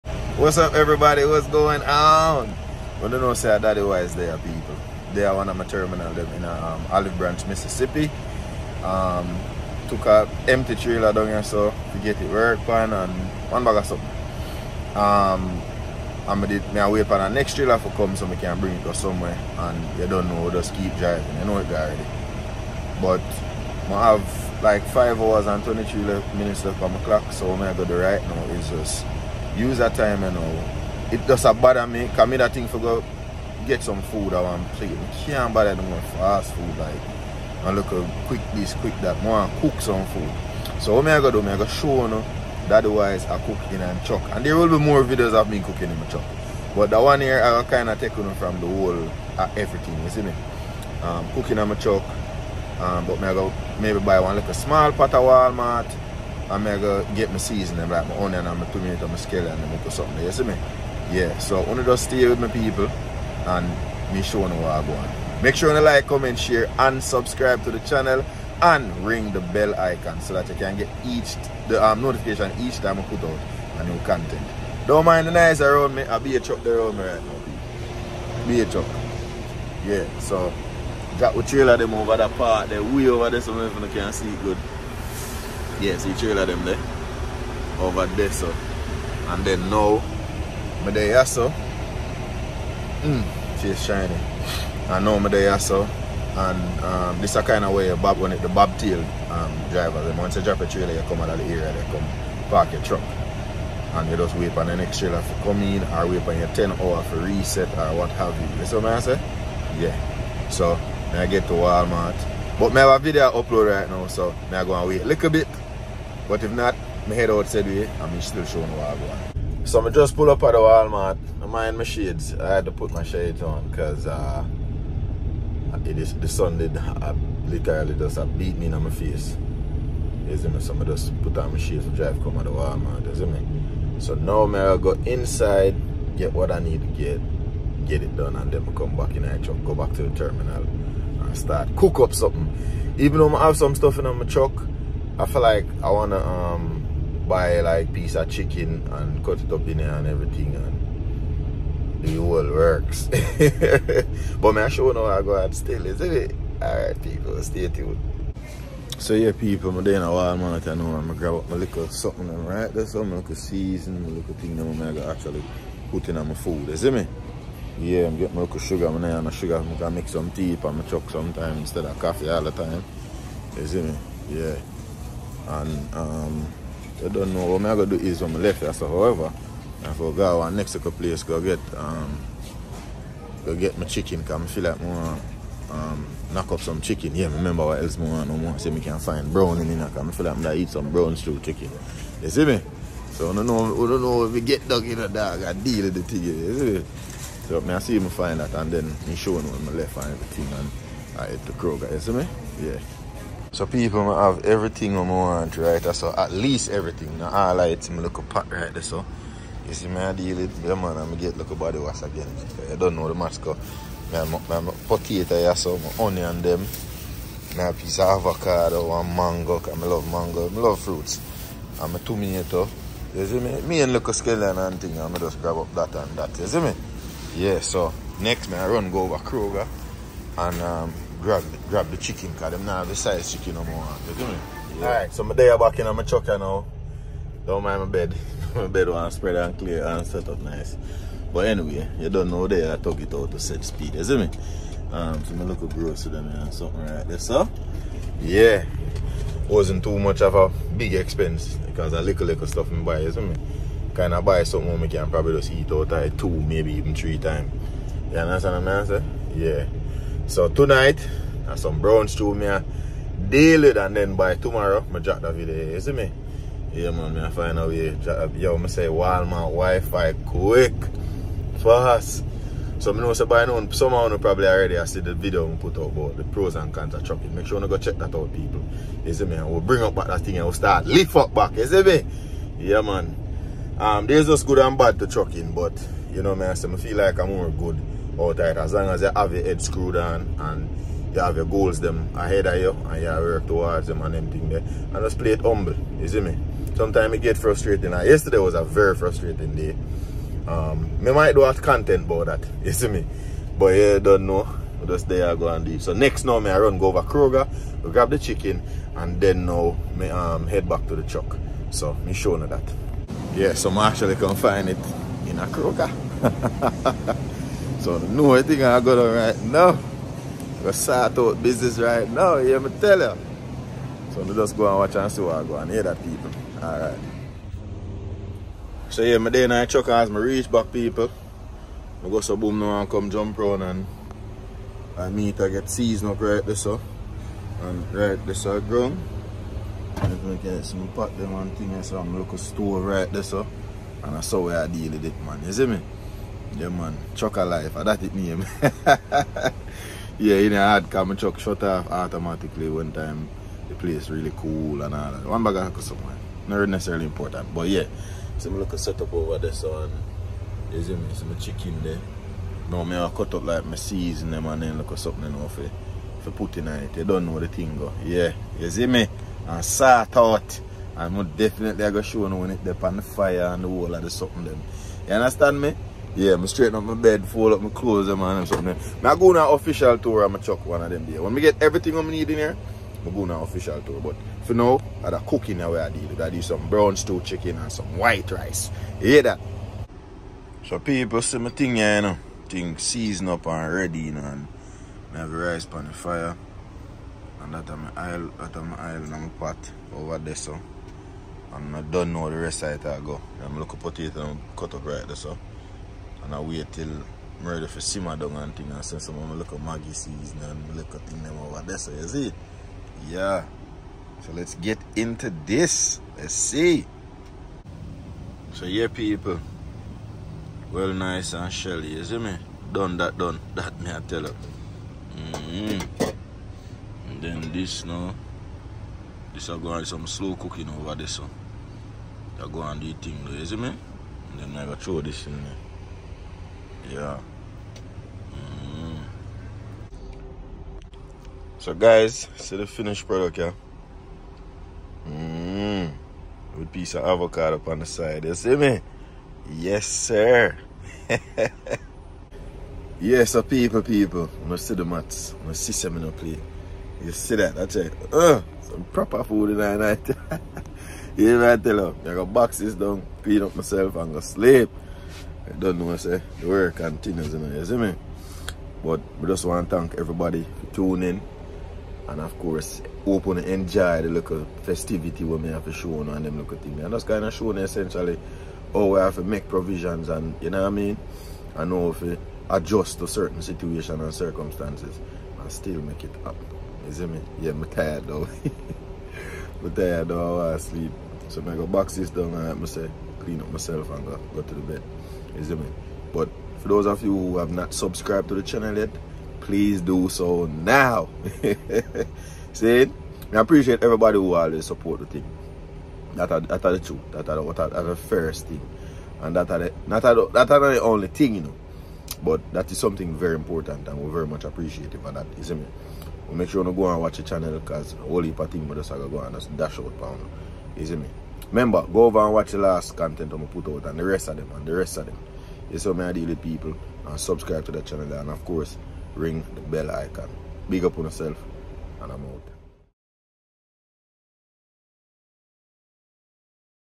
First Time Cooking Jamaican Food In My Truck
Thank you guys for the support!! We appreciate EVERYONE!

Our Family Amazon Wishlist